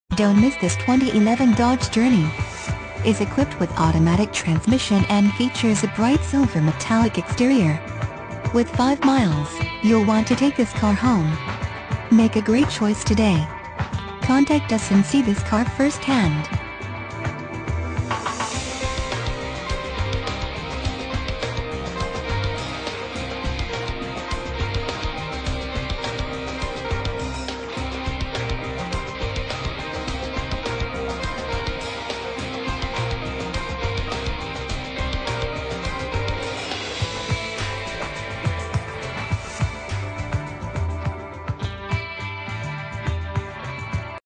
2011 DODGE JOURNEY Denver, CO D11095
2011 DODGE JOURNEY Denver, CO
Stock

Pro Chrysler Jeep
1800 W 104th Ave

Don't miss this 2011 Dodge Journey. It's equipped with automatic transmission and features a Bright Silver Metallic exterior. With 5 miles, you'll want to take this car home. Make a great choice today.